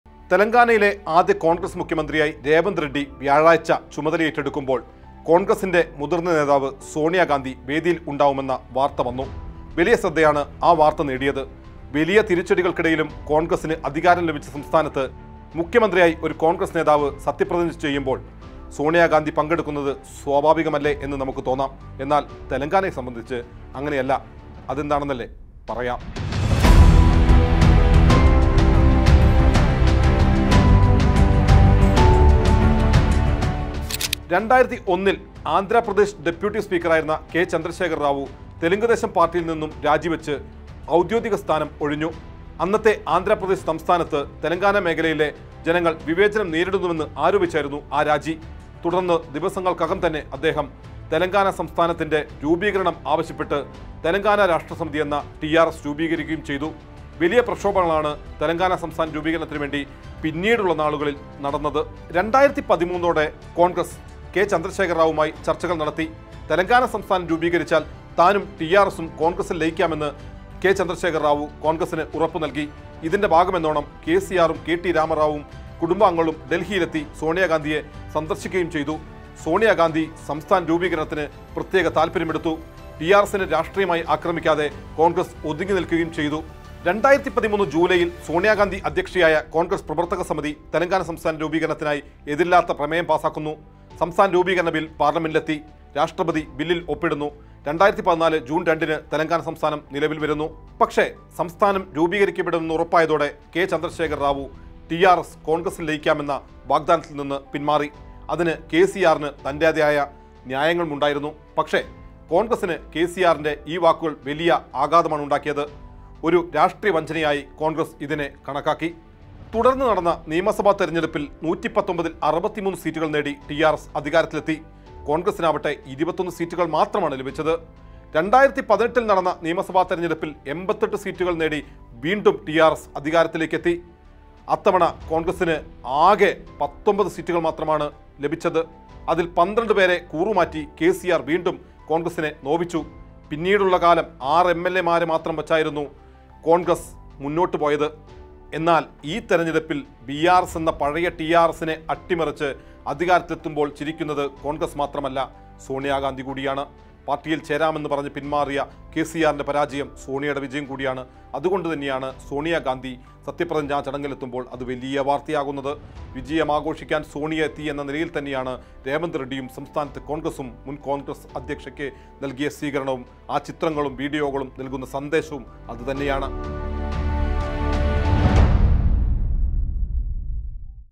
ഇത് സോണിയാ ഗാന്ധിയുടെ പ്രതികാരം | Sonia Gandhi | Telangana | Congress
പത്ത് വര്‍ഷത്തിന് ശേഷം തെലങ്കാനയില്‍ കോണ്‍ഗ്രസ് അധികാരത്തിലേറുമ്പോള്‍ ചിരിക്കുന്നത് സോണിയാ ഗാന്ധി

Sonia Gandhi laughs as Congress comes to power in Telangana after 10 years

With Regards
Team RBC